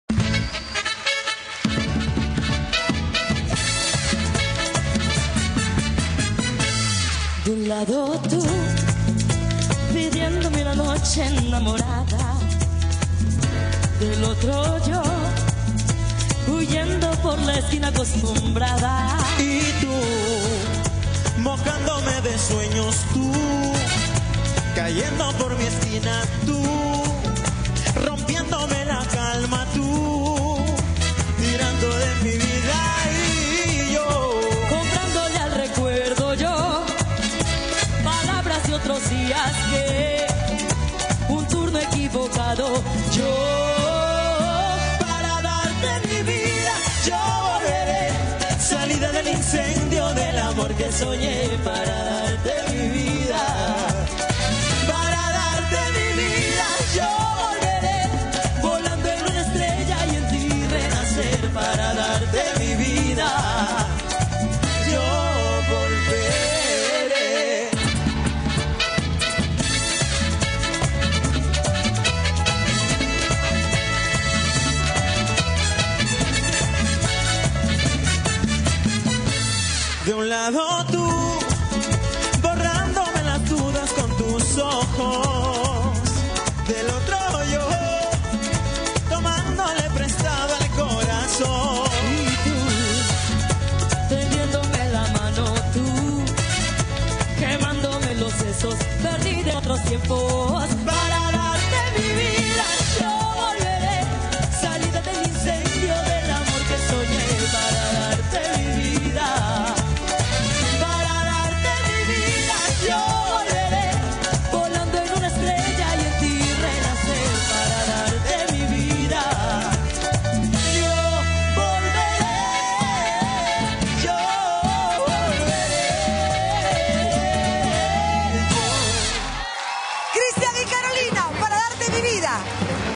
KAROLINA CRUZ 8VA FECHA
KAROLINA CRUZ  8VA  FECHA, UNA SEMANA MAS QUE NUESTRA REPRESENTANTE DE IQUITOS QUE CONTINUA ADELANTE, EXITOS AMIGA SI SE PUEDE.